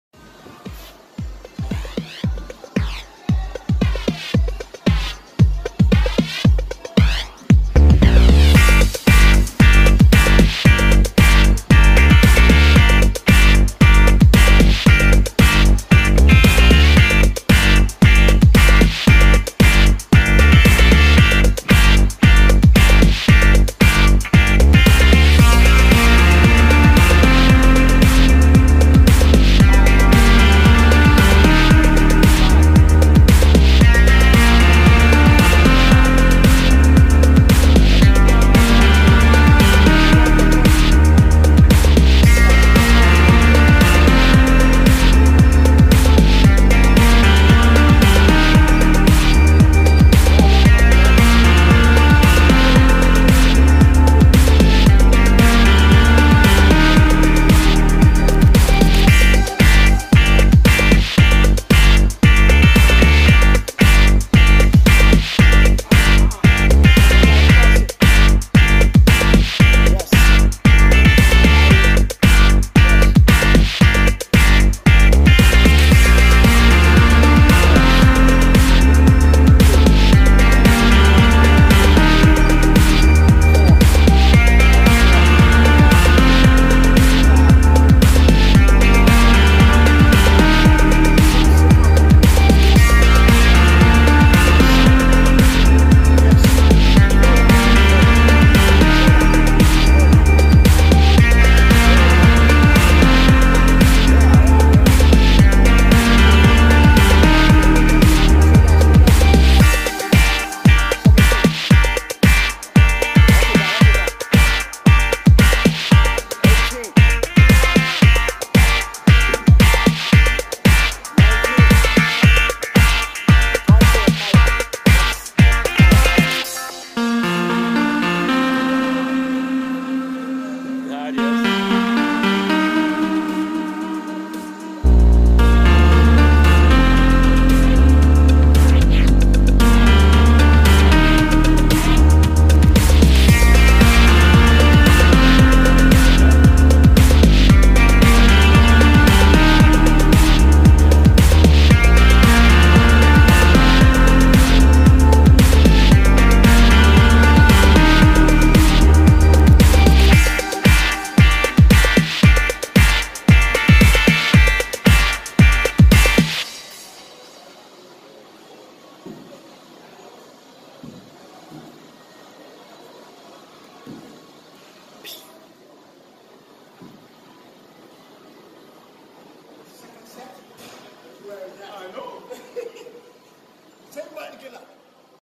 do this workout for BIGGER CHEST
I'm Bruce gunz , this is a very affective compound exercise targets your pecks, triceps, shoulders and core.

if you're an advanced calisthenic or gymnast looking for gains this is the workout for you.

don't jump into this if you're a beginner you could hurt yourself.
 for info on how to prepare for this workout and many more.
INBOX me on IG- BRUCE_GUNZ